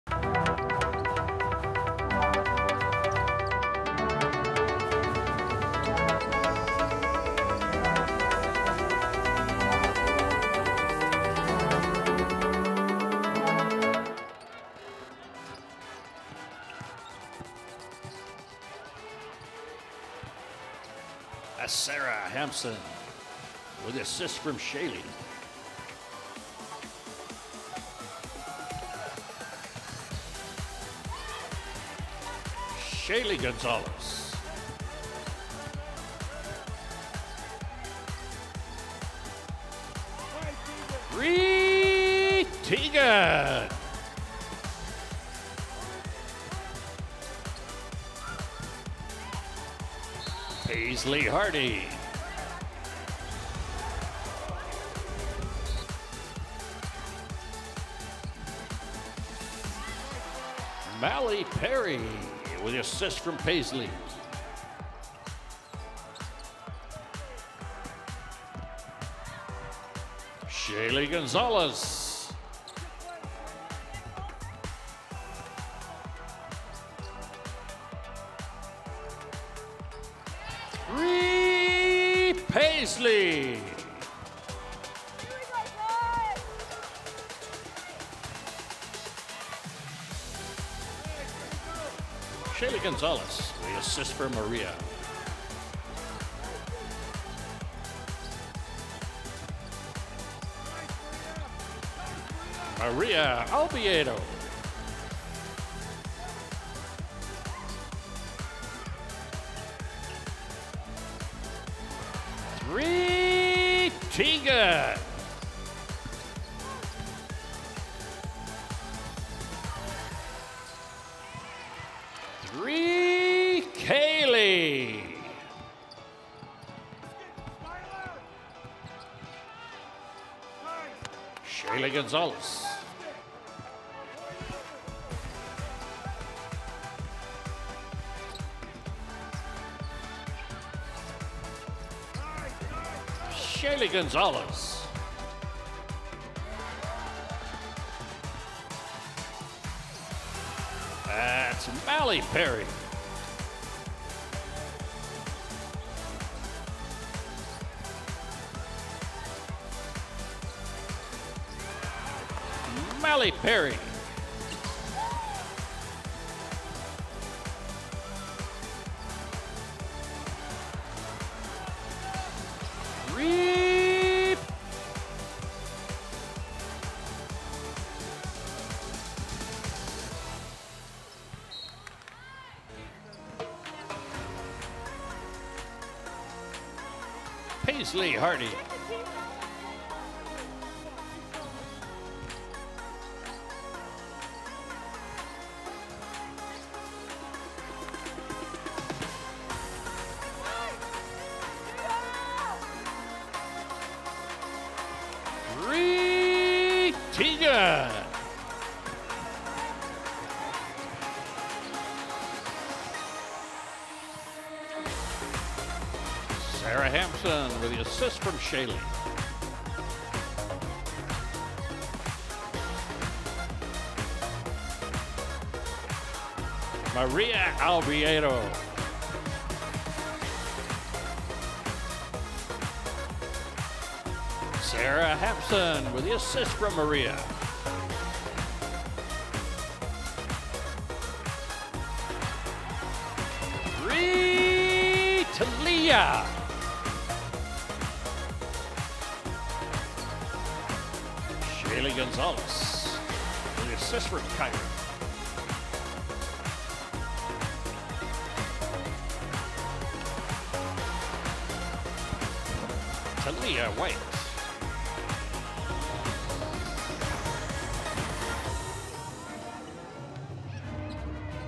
BYU Women’s Basketball | Pacific | Highlights | January 26, 2021
Highlights from the BYU Women’s Basketball 81 to 60 win at Pacific. @shayleegonzales1 with 23 points, 7 rebounds, 6 assists, and 6 steals.